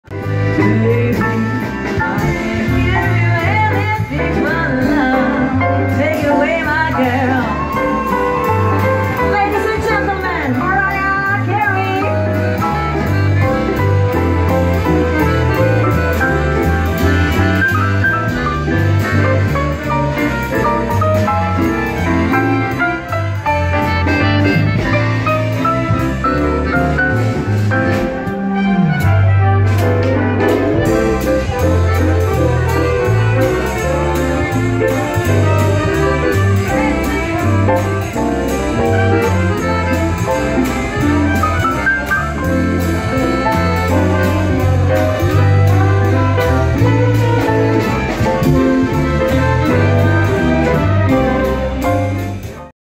Petrica Andrei Orchestra feat. MARIAH CAREY
A small sequence where the famous singer Mariah Carey sang with Petrica Andrei Orchestra!